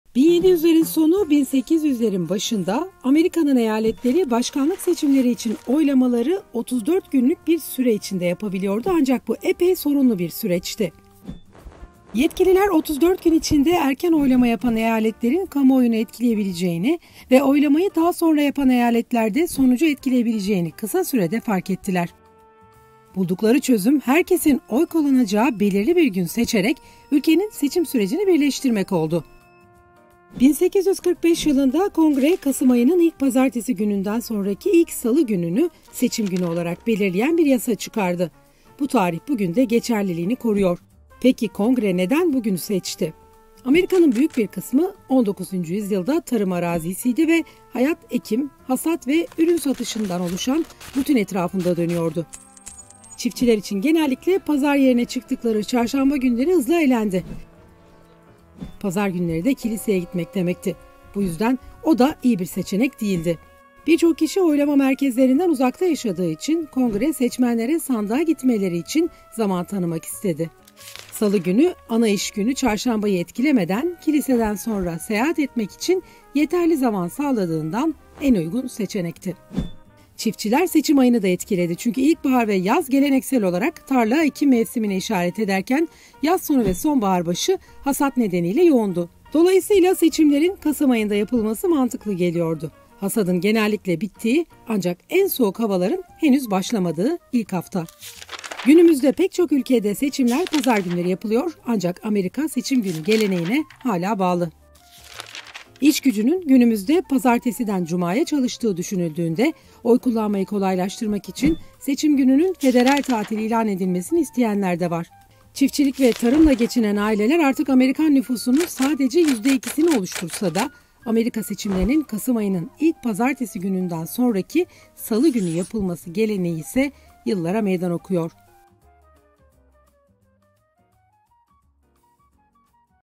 ABD'de seçimler neden hep salı günleri yapılıyor? İşte cevabı!
ABD seçimleri neden salı günü? Dünyanın gözü kulağı ABD seçimlerindeydi. Trump 47. Başkan oldu. Peki seçimler neden salı günü? VOA Türkçe derledi! Keyifli seyirler!

Videoları kullanmamıza izin verdikleri için Voice of America'ya (VOA) teşekkür ederiz!